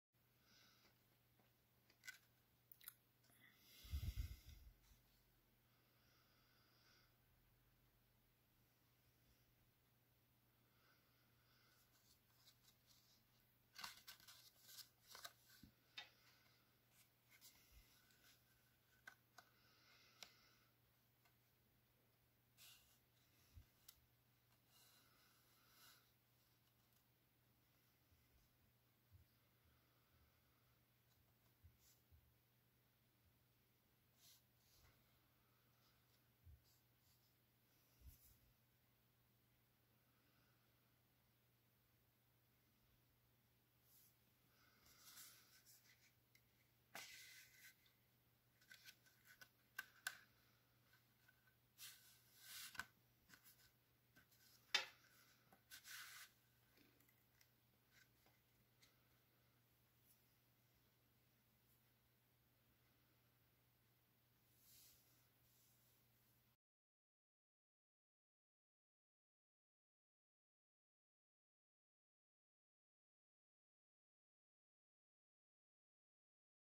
Window slider card with Lavinia stamps.
This WINDOW SLIDER CARD is easy to make,
but the stamping took some more time...
I like to show a little fairy tale with these stamps.